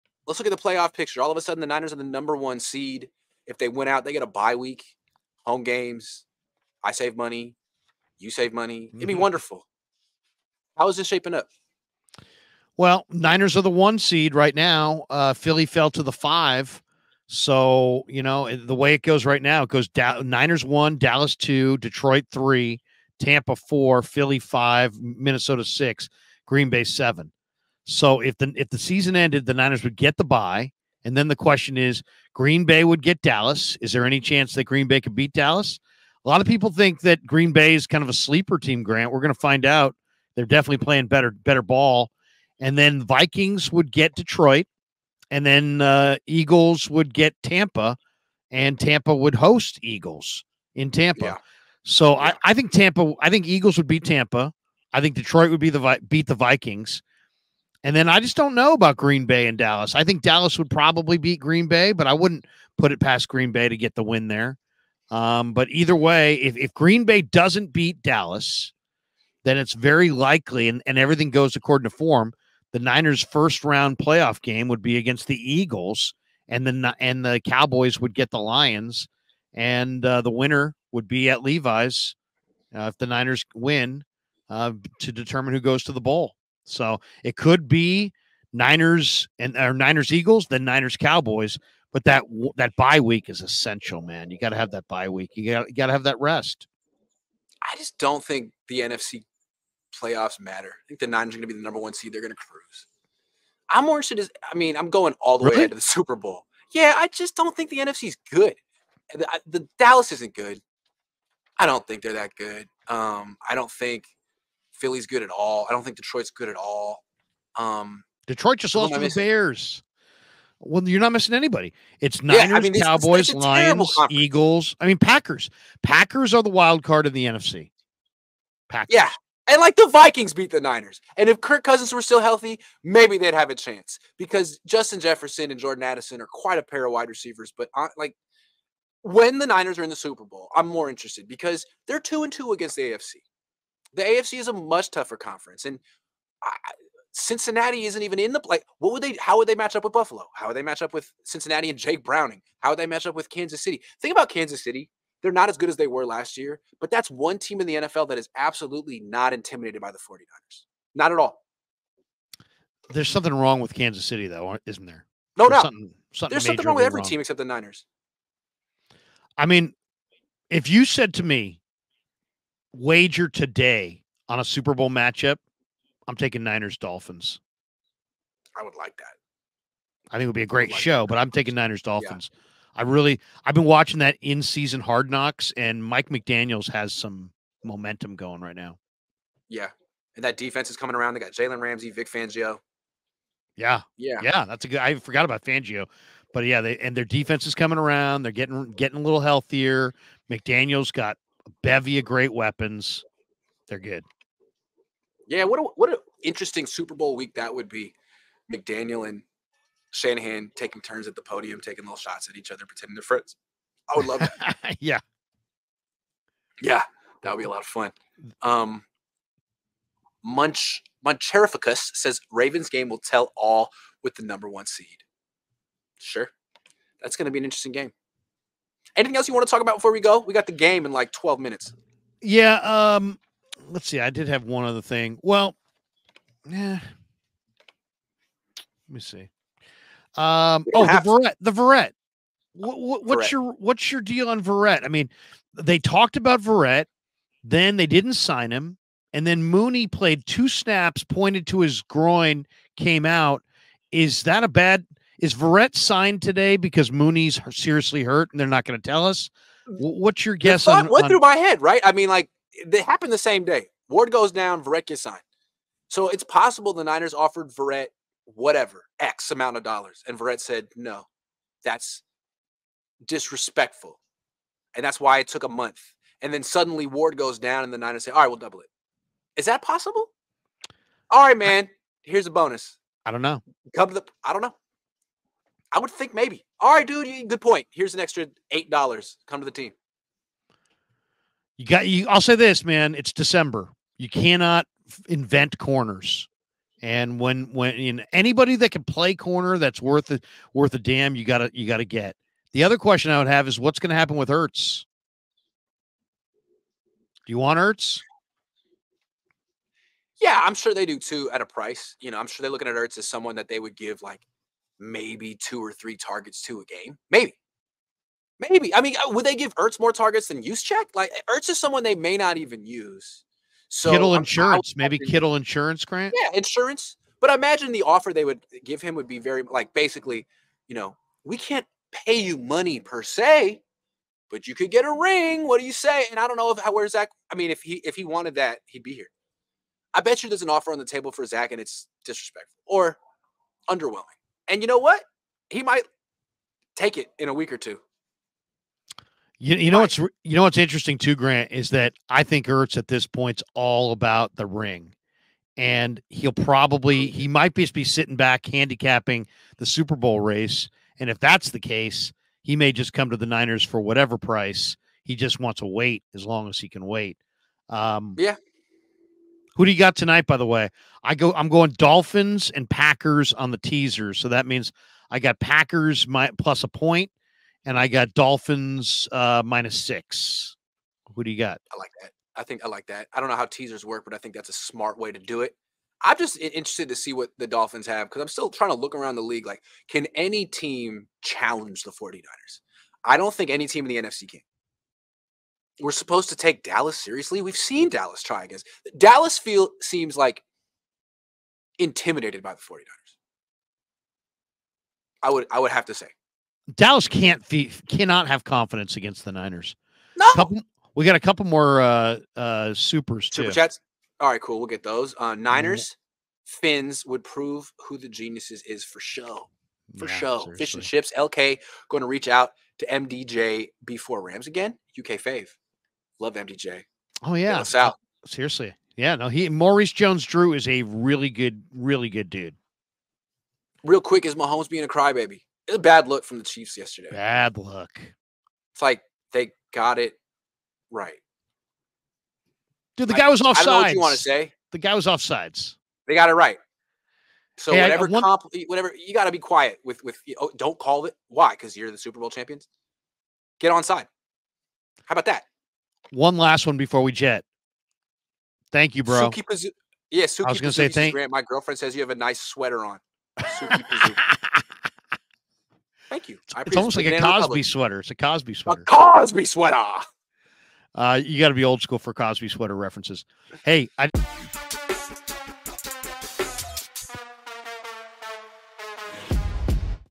How the 49ers Fit in the NFL Playoff Picture
Grant Cohn and Larry Krueger discuss how the San Francisco 49ers fit in the NFL playoff picture.